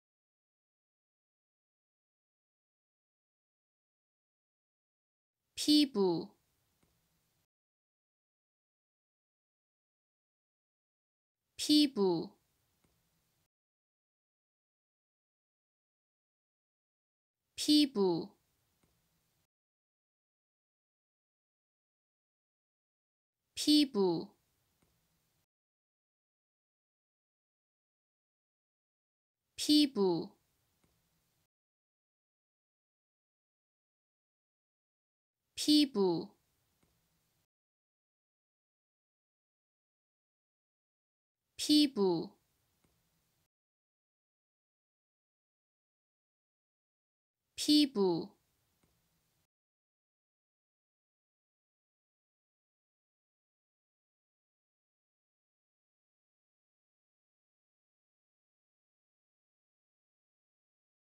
How to say Skin in Korean / 피부 발음
Heyo!

Learn how to pronounce “skin” in Korean.